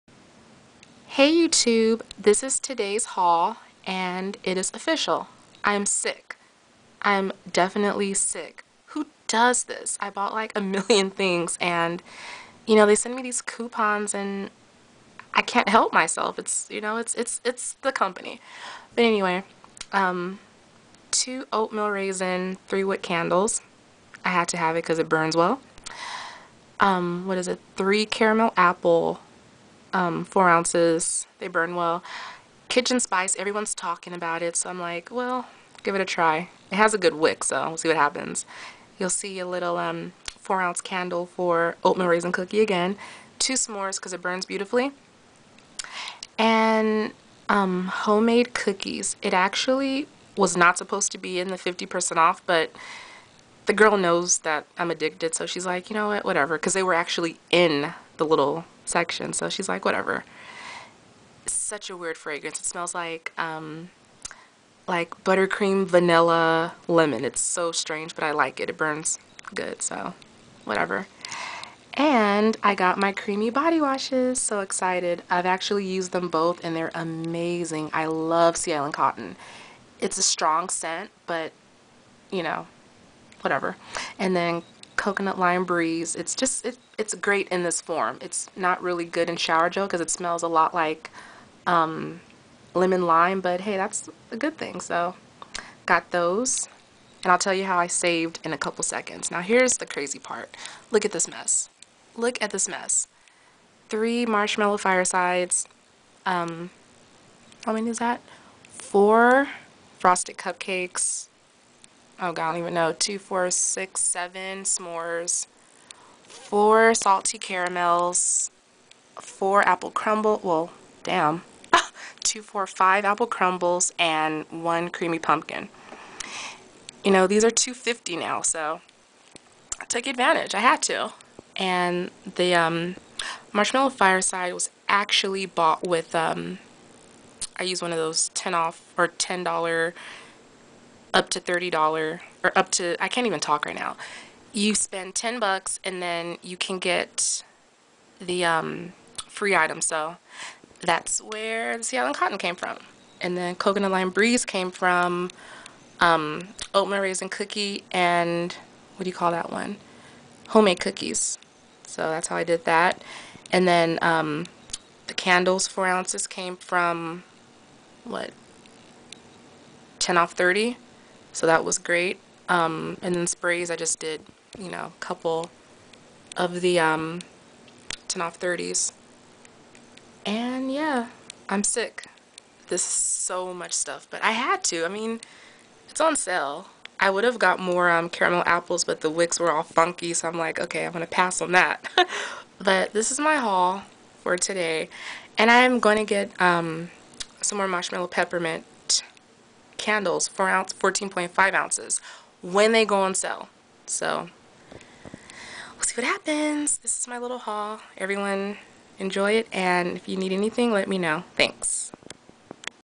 Bath and Body Works Haul Review 50% SALE! 9-9-11
I told myself that I wasn't going to go into B&BW until another 2 for $20 candle sale, but I just couldn't help myself. My fellow B&BW addicts know exactly what I am talking about... Check it out! ♥